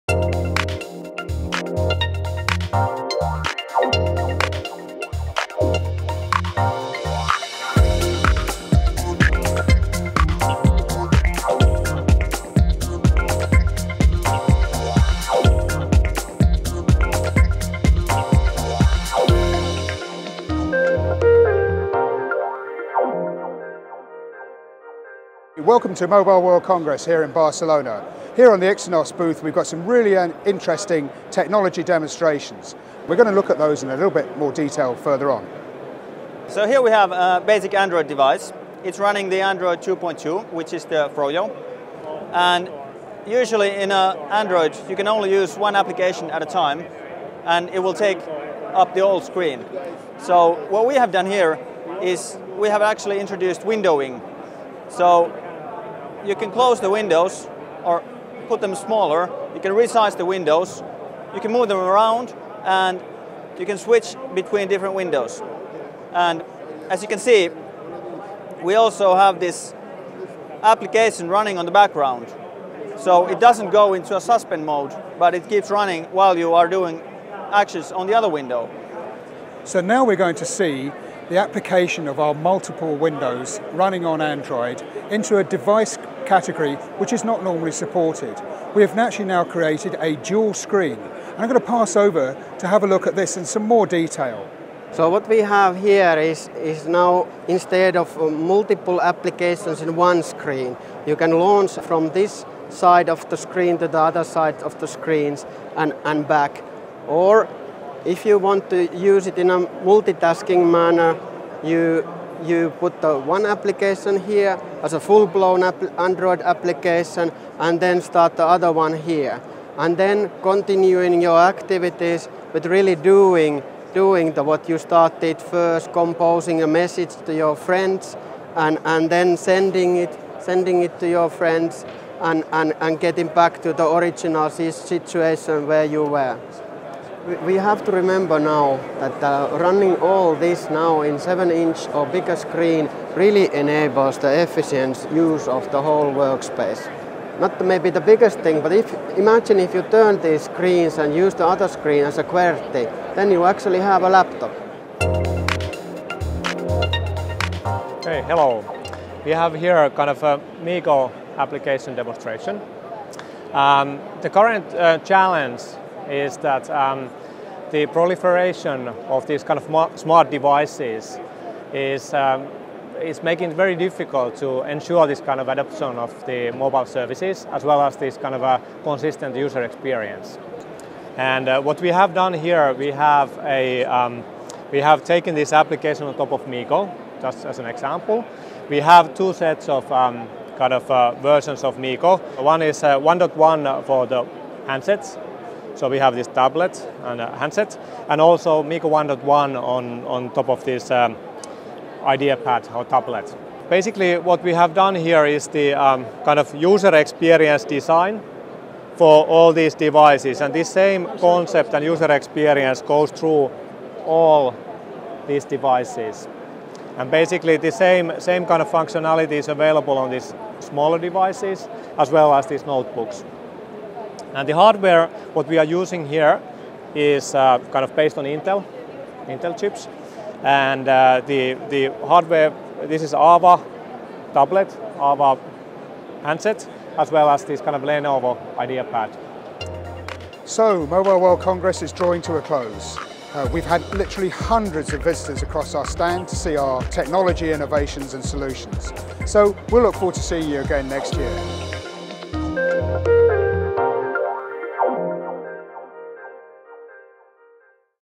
Ixonos showcasing extensive MeeGo & Android expertise at MWC11
Ixonos, a world leader in creating wireless technologies, software and solutions for mobile devices and services, showcased inspiring user experience designs on innovative solutions for Android and MeeGo platforms at the Mobile World Congress, held in Barcelona on 14-17 February 2011. Check out the demonstration of an Android-based multi-window solution running on a tablet device as well as on a dual-screen configuration. The video also holds a demonstration of an end-to-end application solution on a MeeGo v1.1. tablet prototype with an Intel® Atom™ processor.